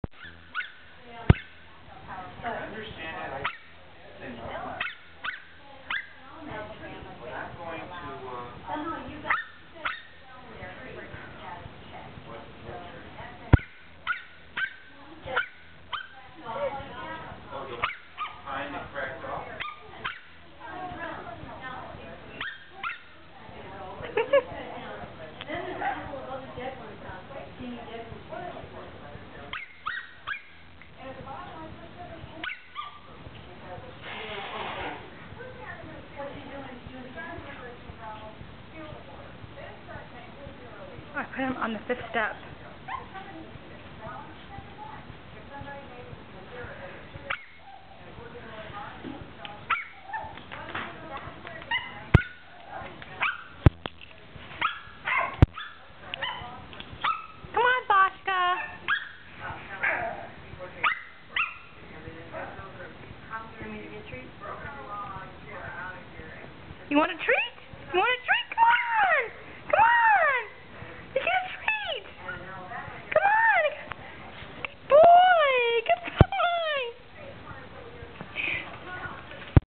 Baška Learning to go down the Stairs!
Baška is a 10 week old Maltese and learning to go down the stairs! He gets a treat for being so brave!!